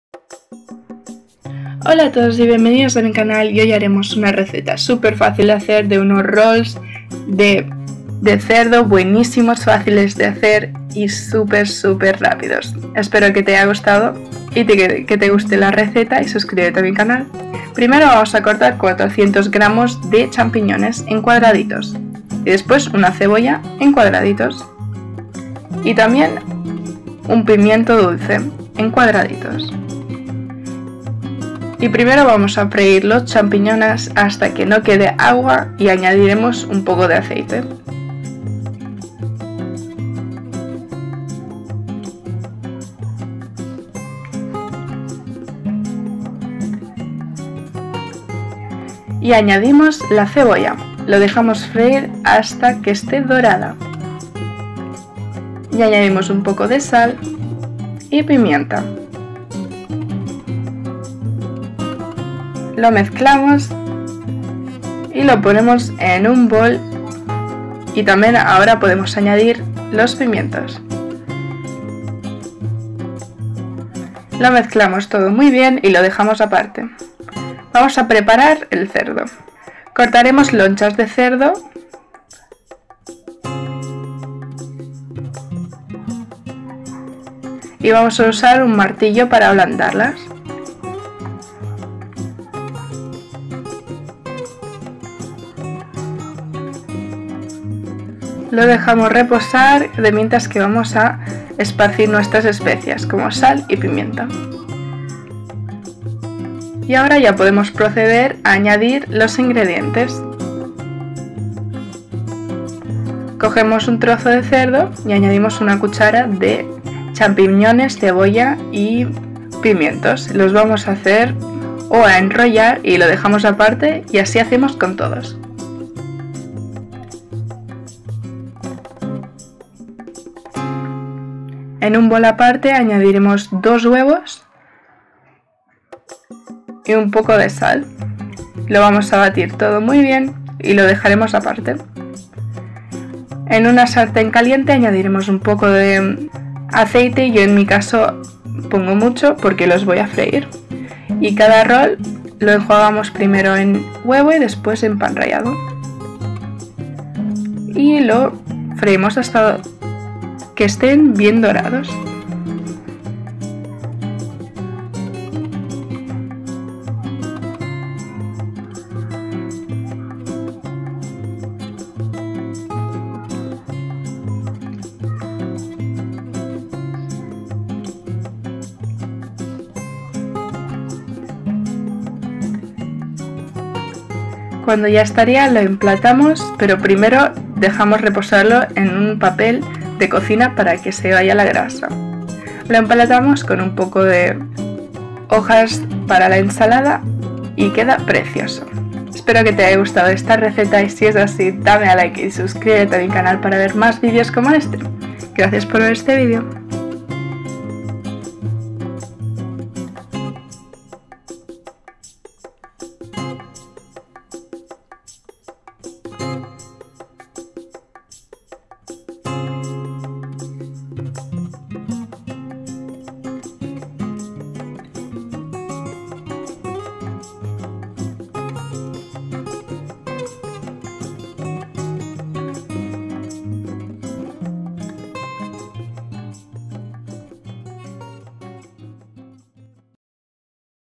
Rollitos de carne con champiñones!Comida casera!
Rollos de carne con champiñones, este es un plato caliente en el que todos ganan. La carne tierna va bien con el relleno de champiñones fragantes. Estos panecillos se preparan de forma muy sencilla y son perfectos como plato caliente en la mesa festiva.
INGREDIENTES:
carne de cerdo (carbonada) - 1 kg
champiñones - 400 g
huevos - 2 piezas
cebolla - 1 pieza
pimiento dulce - 1 ud.
pan rallado - 150 g
aceite vegetal
sal al gusto
pimienta negra molida - al gusto